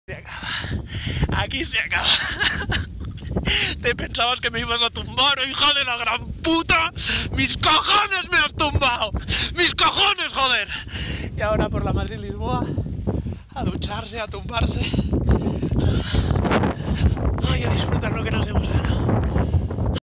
MONGOLIA LLEGADA!!!
YA SON NUESTROS! 900KM a la saca después de mucho sufrir, pedalear, llorar y sobre todo, disfrutar de lo vivido!